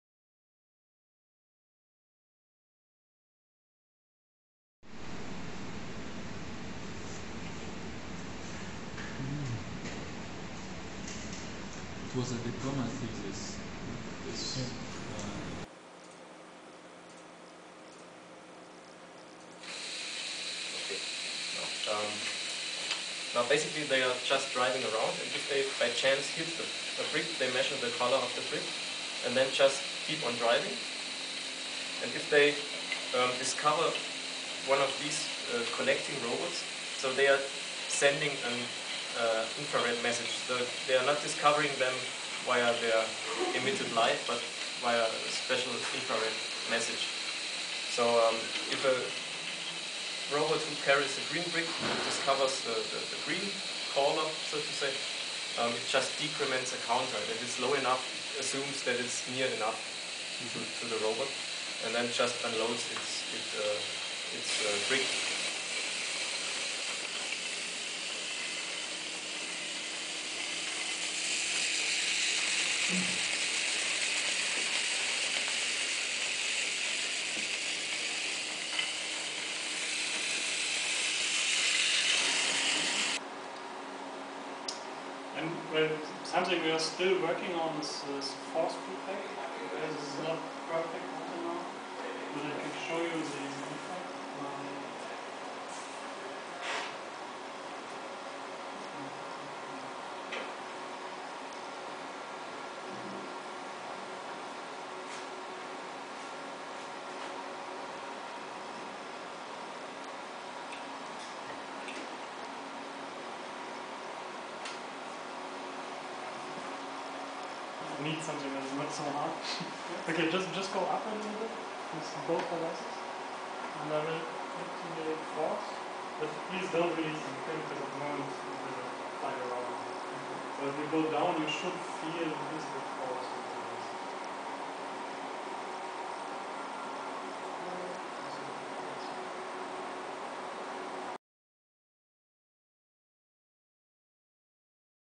A visit to Karlsruhe Institute of Technology on 2011-04-01
This video describes some of the current work in robotics being undertaken at the Karlsruhe Institute of Technology in Germany. It shows several demonstrations organised by KIT during a visit from a UNSW Ph.D student. Thanks to Wilhelm August at KIT for organising the visit and demonstrations.